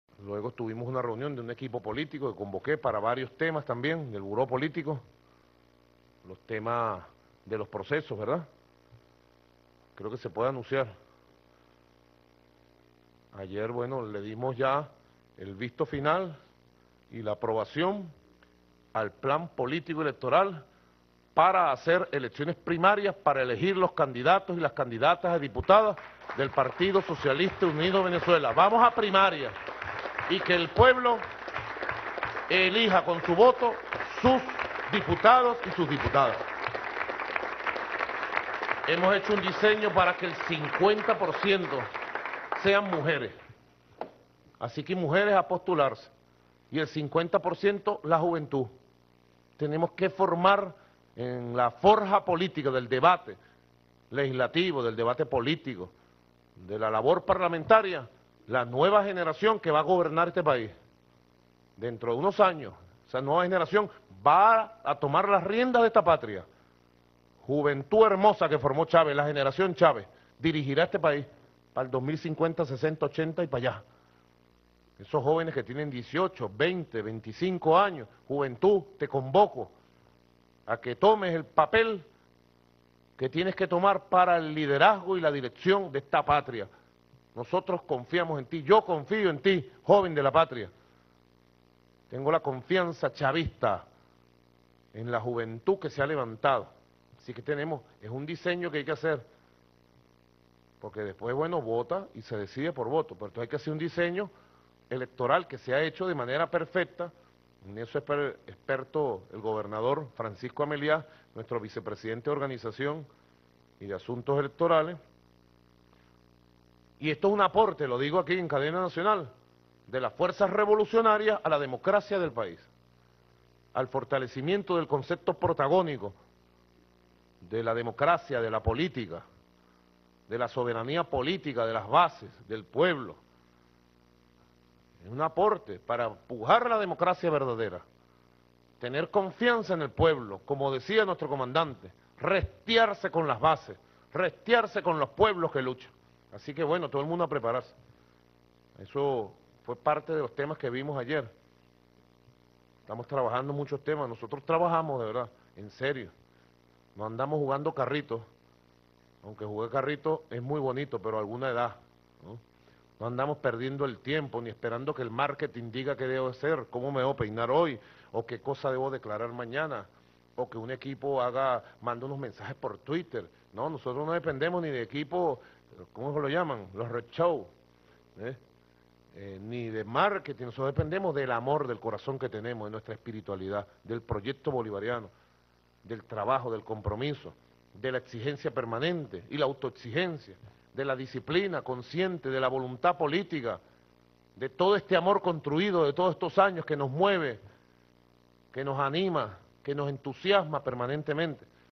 Maduro anuncia que habrá primarias en el PSUV para elecciones diputados 2015
Anuncio hecho este 4 de enero de 2015 desde el Palacio de Miraflores. El Presidente venezolano, Nicolás Maduro, también anunció que el 50 por ciento de los candidatos a diputados del partido serán mujeres, y el 50% serán jóvenes.